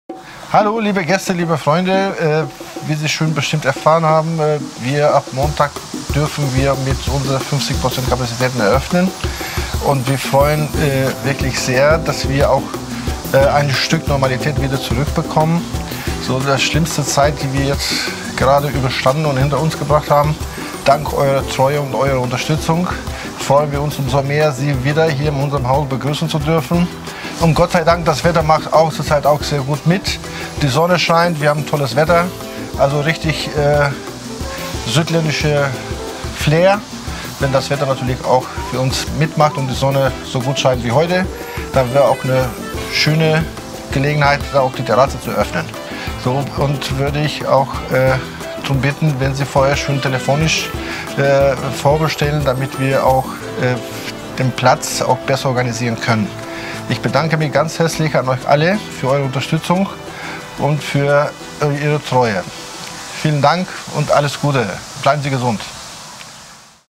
Restaurant Helena
Nach 8-wöchiger Schließung nun endlich wieder geöffnet. Unbedingt vorher einen Tisch reservieren...